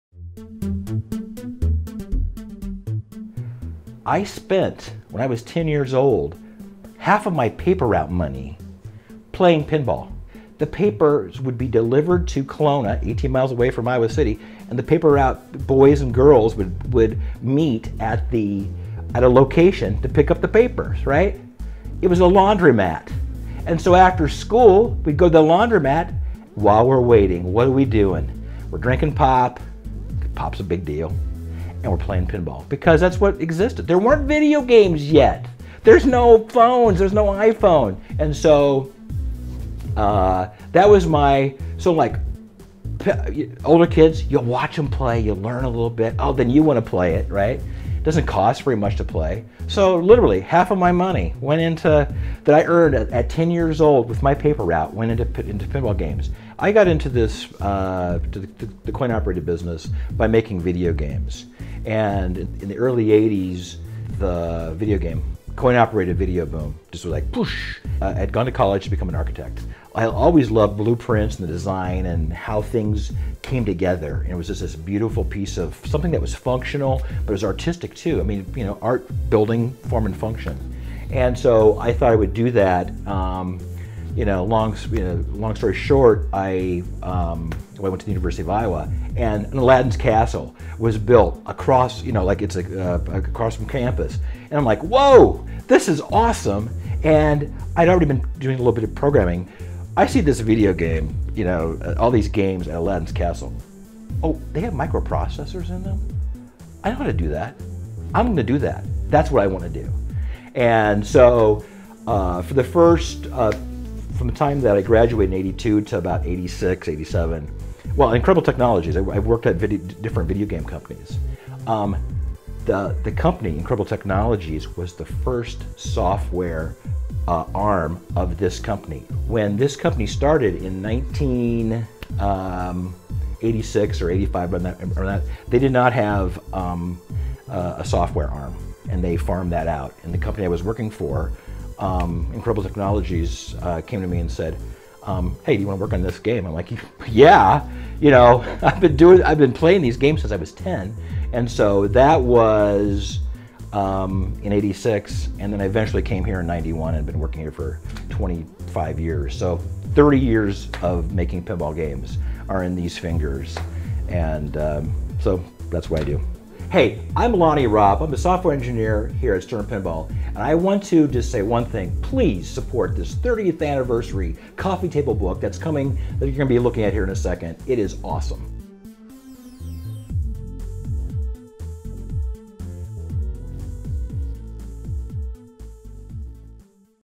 Lonnie Ropp talks about how he got into Pinball
Please support: Keeping the Ball Alive: 30 Years of Stern Pinball on Kickstarter: